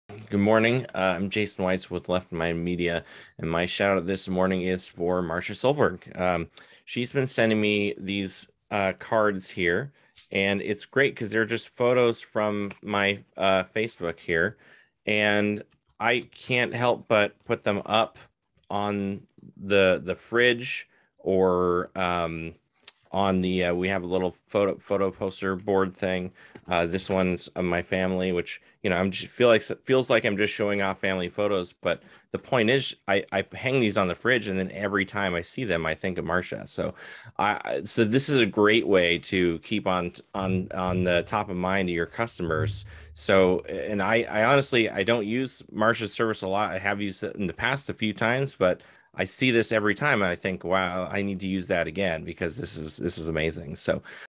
Testimonial for Send Out Cards with Marcia Solberg
Testimonial videos are recorded at Good Morning Tigard by the Tigard Chamber of Commerce.